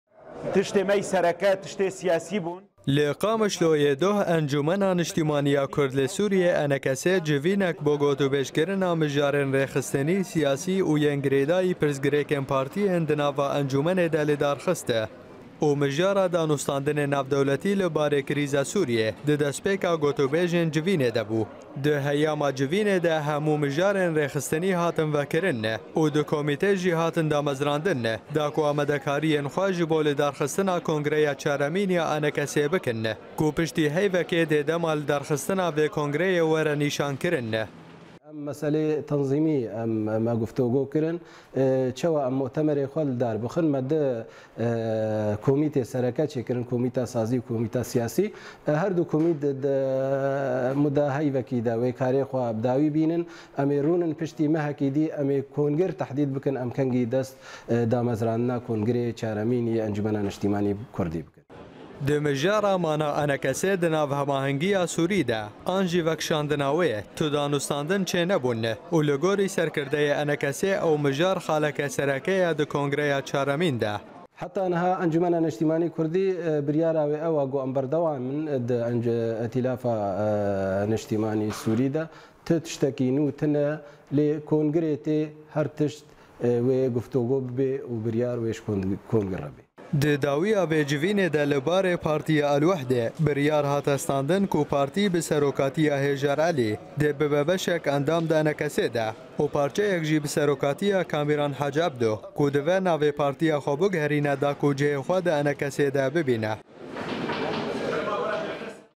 ENKSê civînek ji bo amadekirina kongireyeke lidarxist
Civata  ENKS Li Qamişlo doh li dar ket, û têde mijarên Siyasî û rêxistinî hatin gengeşe kirin, herwiha hin pirsgirêkên parçebûna partiyên ENKS hatin çareserkirin û xalên sereke yên vê civînê hatin Şirovekirin